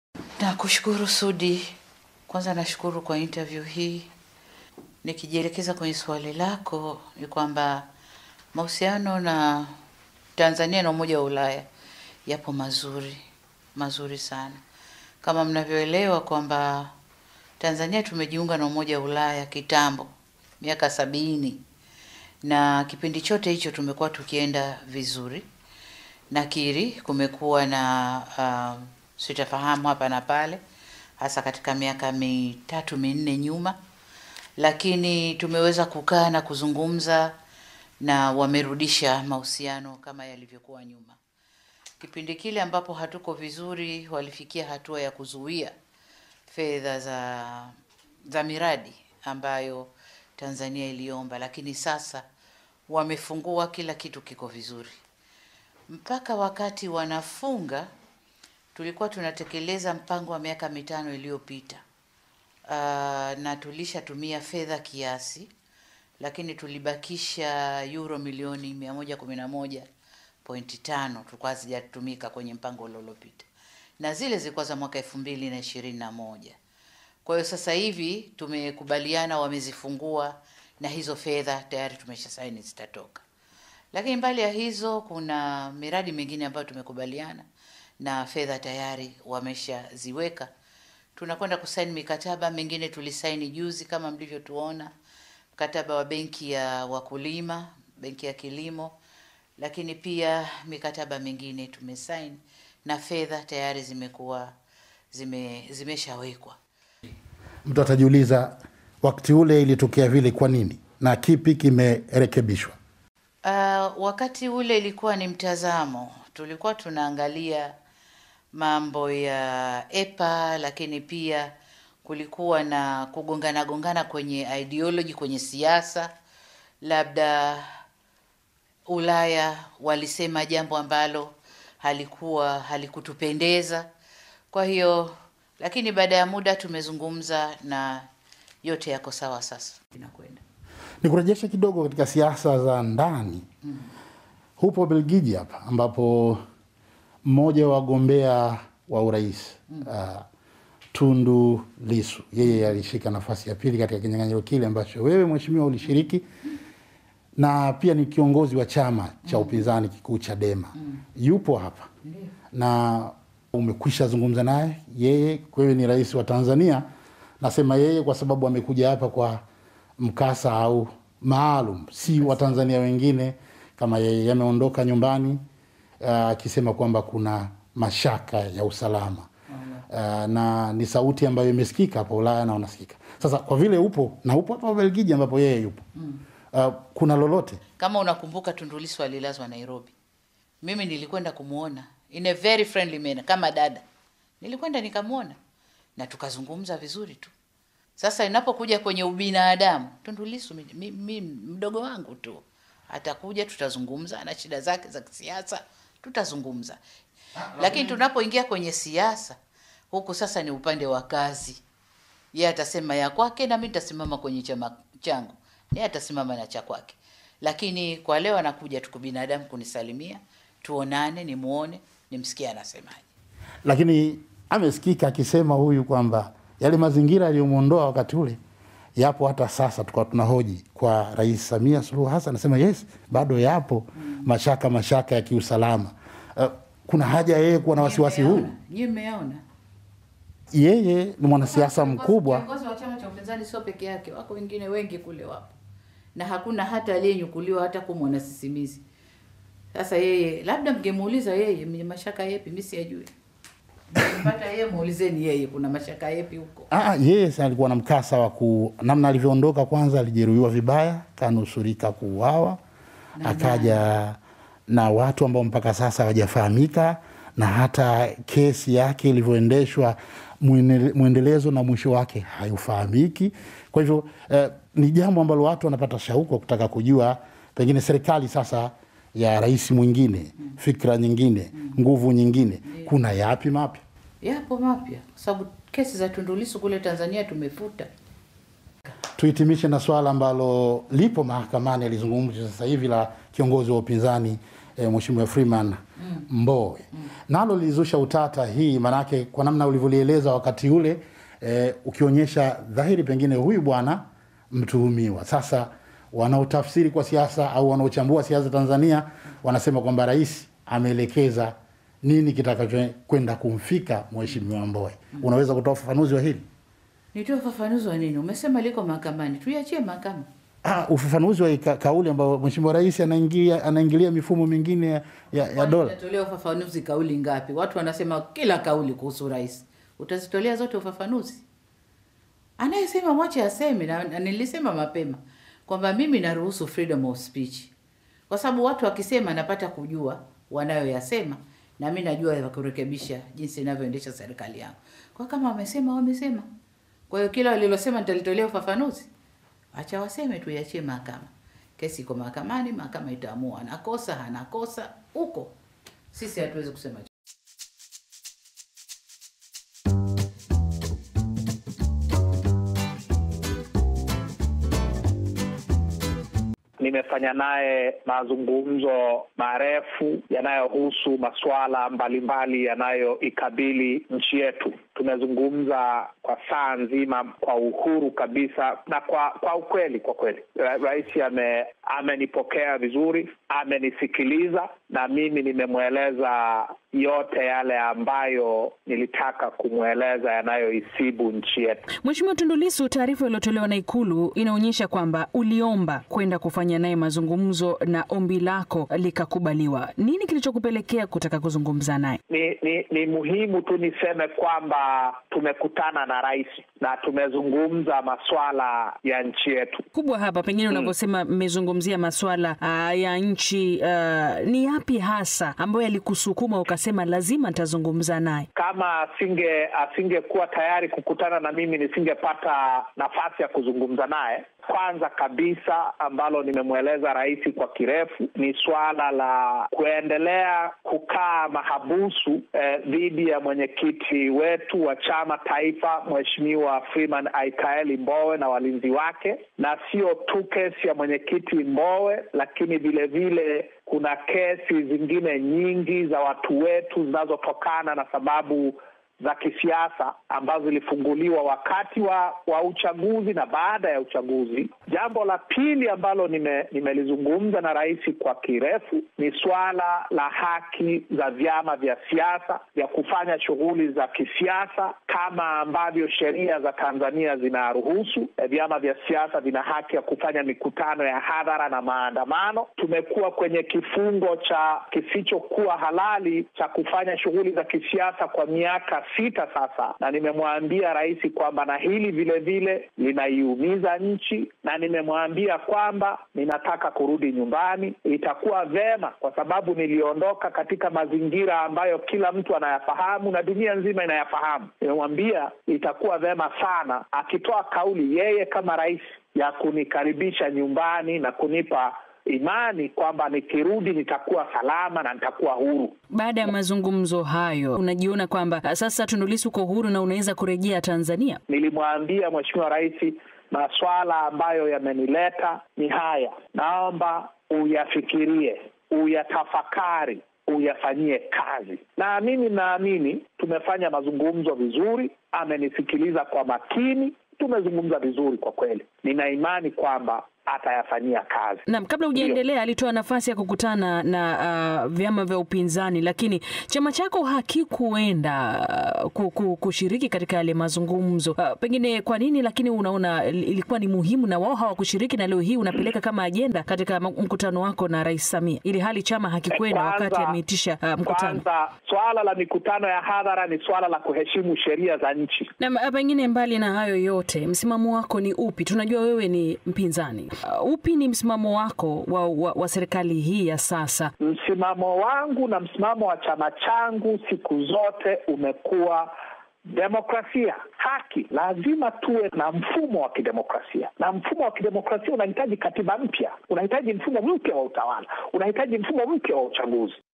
Mahojiano na Rais Samia Suluhu, agusia uhusiano wa Tanzania na Ulaya, kesi ya Mbowe, kikao na Lissu
Mahojiano na Rais wa Tanzania, Samia Suluhu Hassan ambaye kwa sasa yuko mjini Brussels, Ubelgiji kuhudhuria mkutano wa kilele kati ya viongozi wa Umoja wa Afrika na Umoja wa Ulaya.
Katika Mahojiano hayo na DW kiswahili Rais amezungumzia uhusiano kati ya Tanzania na Ulaya, kesi ya kiongozi wa chama kikuu cha upinzani nchini Tanzania, Freeman Mbowe na mazungumzo yake na makamu mwenyekiti wa Chadema, Tundu Lissu.
Wakati huo huo Tundu Lissu ameweka wazi mazungumzo yake na Mh Rais Samia. Huku akisisitiza maswala ya Demokrasia na Kesi ya Mbowe.